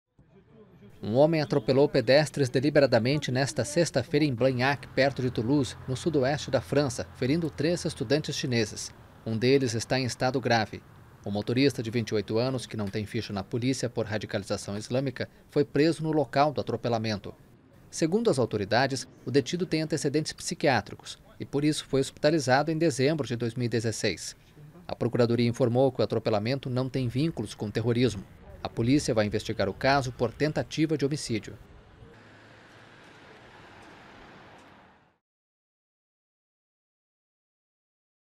Homem atropela pedestres no sudoeste da França
Três pessoas foram atropeladas por um homem que dirigia um carro na França. O estado de um dos feridos é grave.